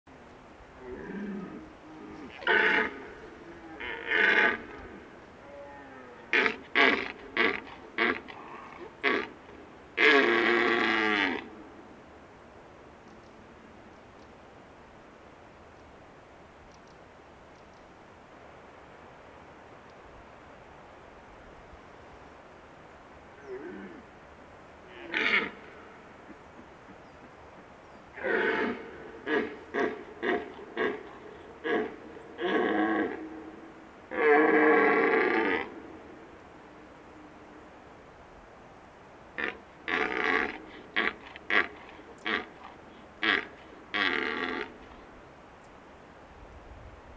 Red stag with face paralysis / Cerb cu pareză facială
Amazing encounter with a big stag with face paralysis. His right side of the face is hanging and this is probably an injury from a fight with another stag.  For the whole story visit the following video on my channel